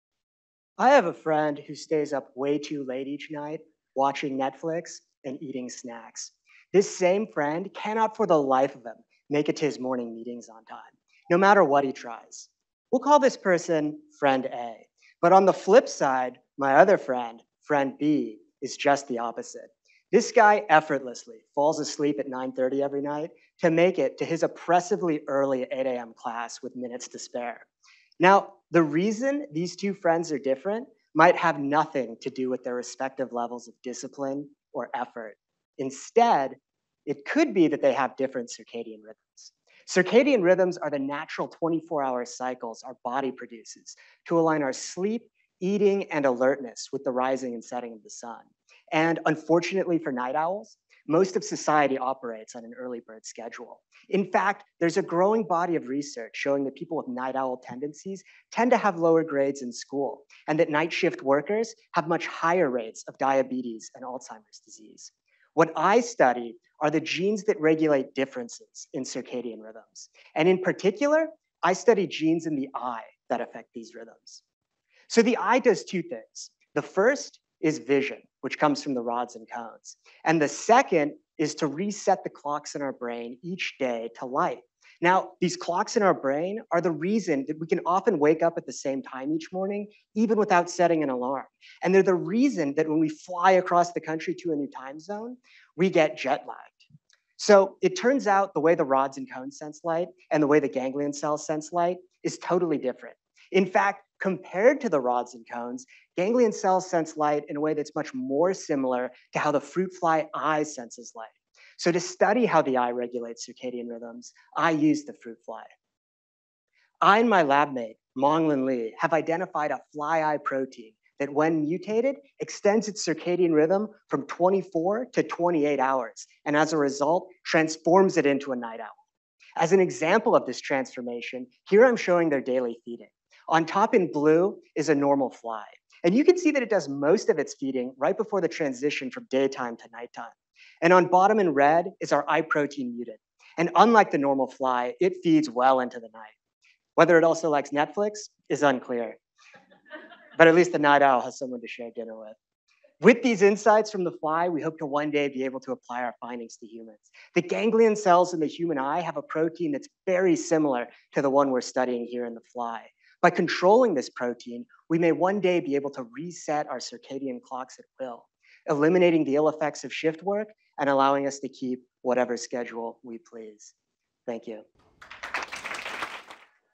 The early bird gets the worm, but the second mouse gets the cheese
UC Santa Barbara grad student Geoff Meyerhof delivers a 3-minute talk at the 2022 UC Grad Slam on his research into the genes that affect circadian rhythms, and a protein in the eye that could help reset our daily clocks.